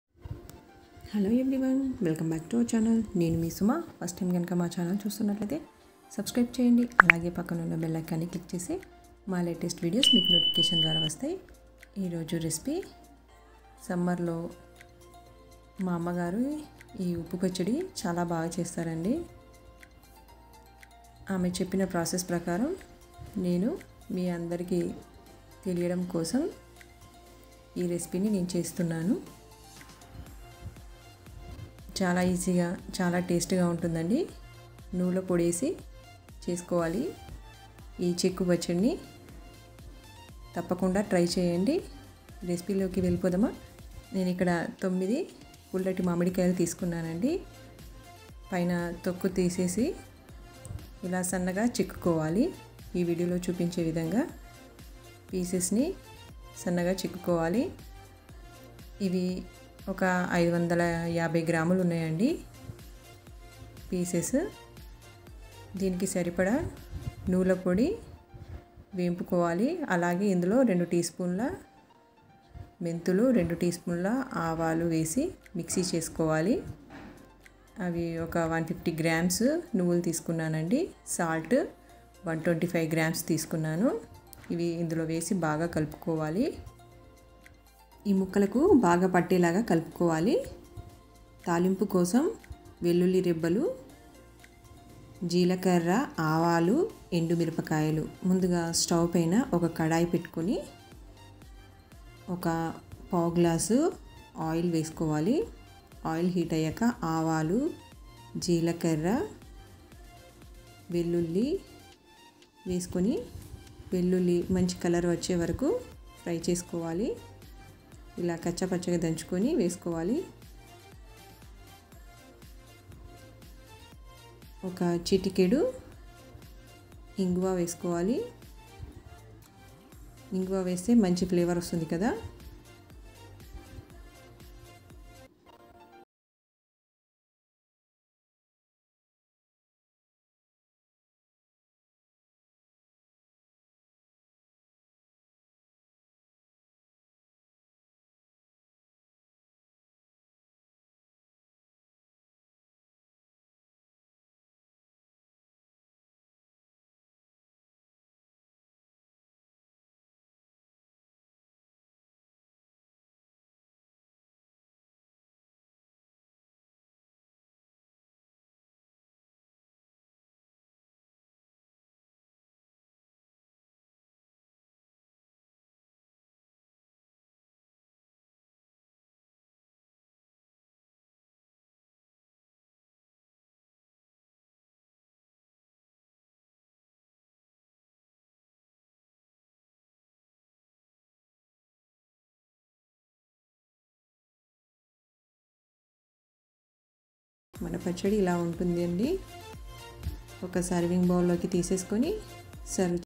మామిడికాయ చెక్కు పచ్చడి నువ్వుల పొడి తో చాలా కమ్మగా వుంటది
today's recipe

మామిడికాయ చెక్కు పచ్చడి

125 gram salt
150 gram sesame seeds
2 tsp aavalu
2 tsp fenugreek seeds
550 gram mamidikaya mukkalu
garlic
popu dhinusulu